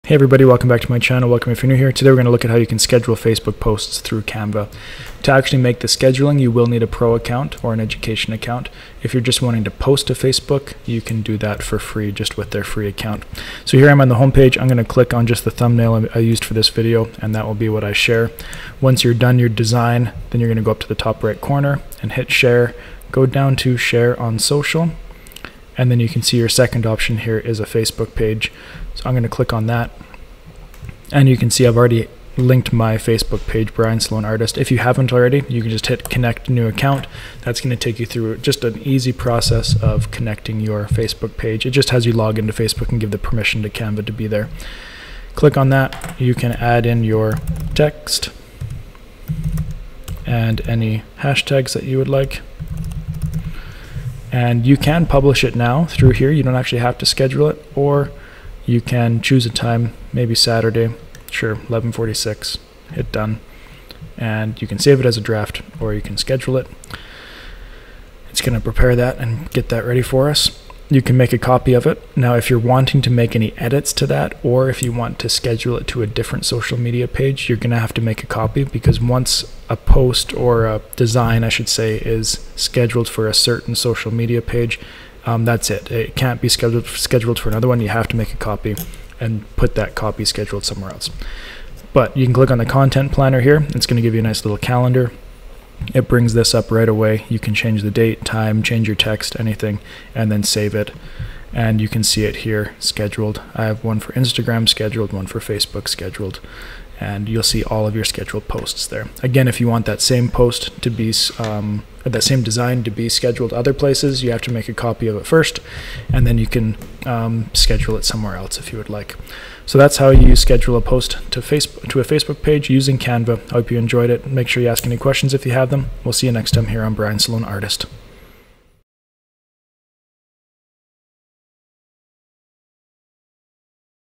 How To Schedule Facebook Posts On Canva
Use Canva to up your social media game by learning how to schedule posts to your Facebook page.

If you enjoyed this video and want to help me continue to make these types of videos, please consider donating to the cause below: